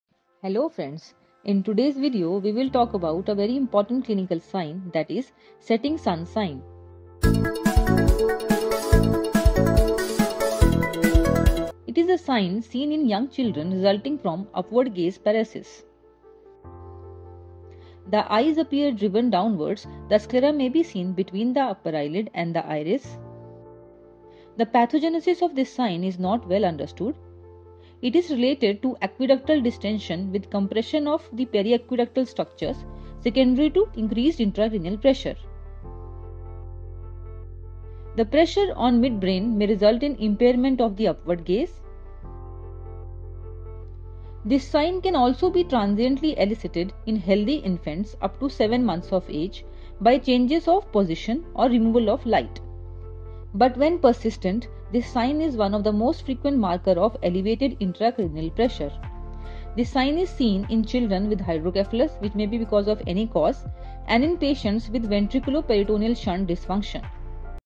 Setting-sun sign | raised intracranial pressure in infants | hydrocephalus
The setting-sun sign  is seen in young children resulting from upward-gaze paresis.

In this condition, the eyes appear driven downward, the sclera may be seen between the upper eyelid and the iris, and part of the lower pupil may be covered by the lower eyelid.

Pathogenesis is not well understood, but it seems to be related to aqueductal distention with compression of periaqueductal structures
secondary to increased intracranial pressure.

it can also be transiently elicited in healthy infants up to 7 months of age by changes of position or removal of light (benign setting-sun phenomenon).

The benign form might represent immaturity of the reflex systems controlling eye movements.

When persistent, this sign is one of the most frequent markers of elevated intracranial pressure.

It is an earlier sign of hydrocephalus than enlarged head circumference, full fontanelle, separation of sutures, irritability or vomiting. Consequently, this sign is a valuable early warning of an entity requiring prompt neuroimaging and urgent surgical intervention.